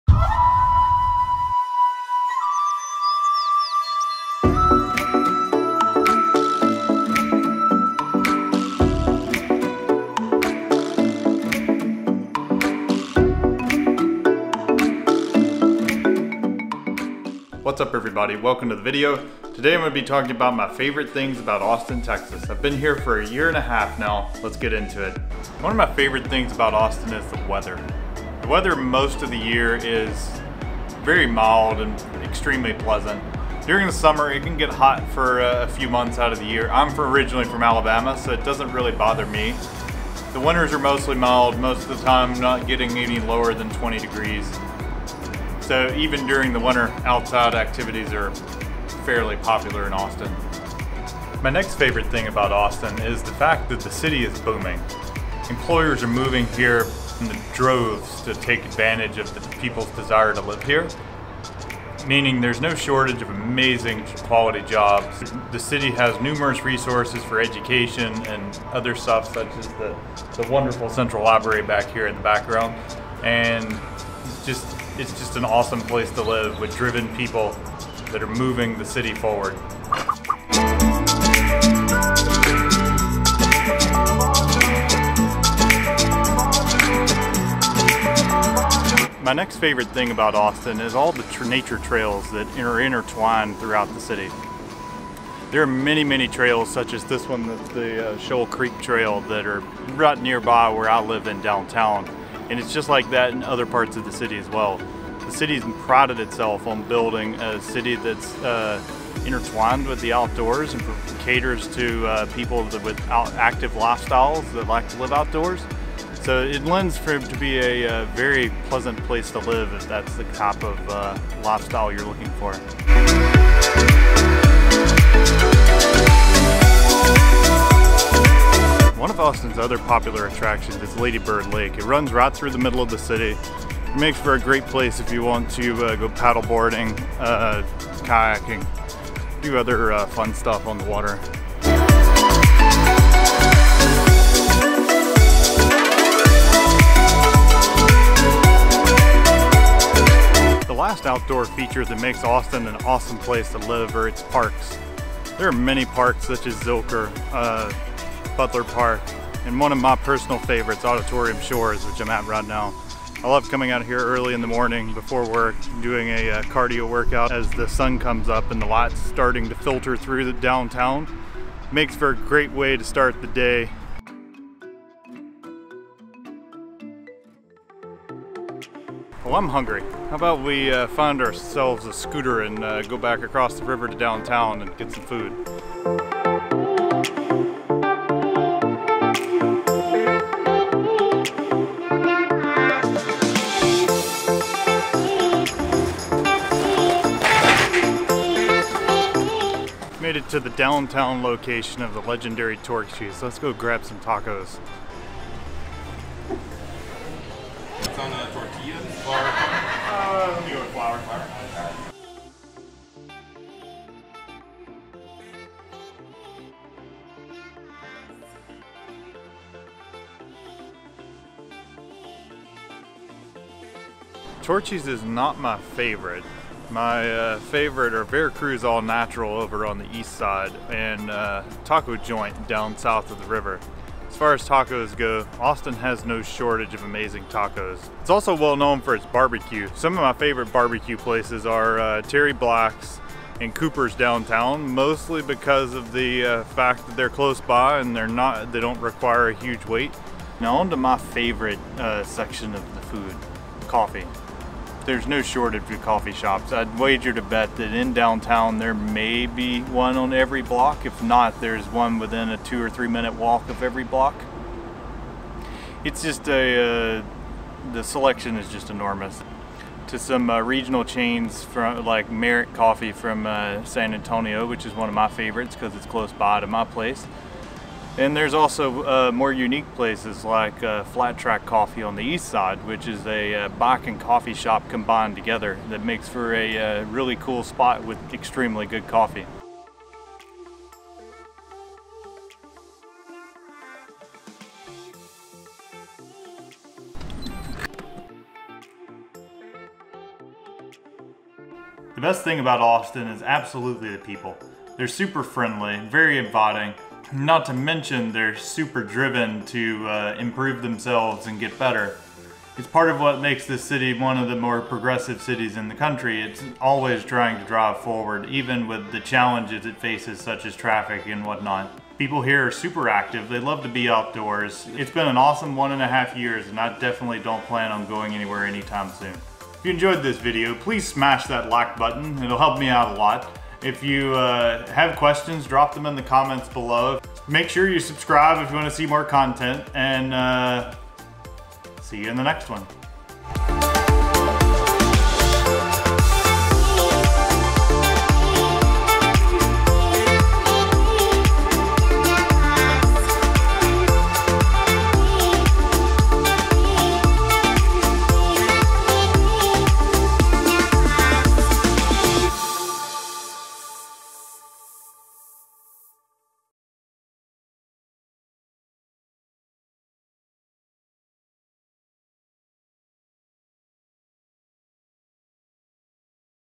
My Favorite Things About Living in Austin | ATX Vlog
I moved to Austin in April of 2018. In this video, I cover my favorite things about living in Austin including my overall favorite. I intended to do this video at the 1 year mark, but it ended up taking me longer to finish that I had hoped. It covers a little bit of everything including the Food, Outdoor, Activities, and other things.

Main Camera (Sony a6400)
Joby GorillaPod Pro
Rode Video Micro
Small Rig Mic Relocator